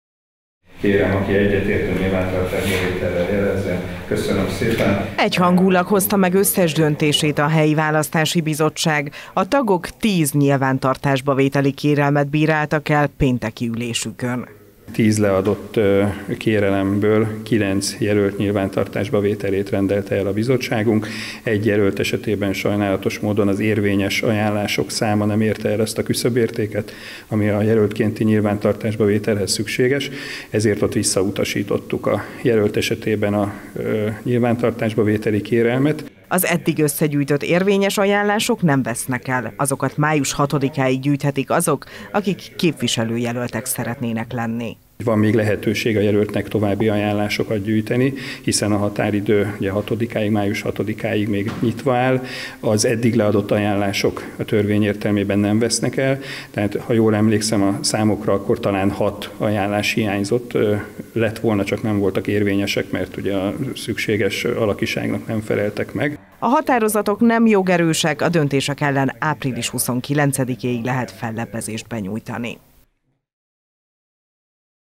Újabb jelölteket vettek nyilvántartásba
Kilenc önkormányzati képviselőjelöltet vett nyilvántartásba pénteki ülésén a Helyi Választási Bizottság. A határozatok nem jogerősek, a döntések ellen április 29-ig lehet fellebbezést benyújtani.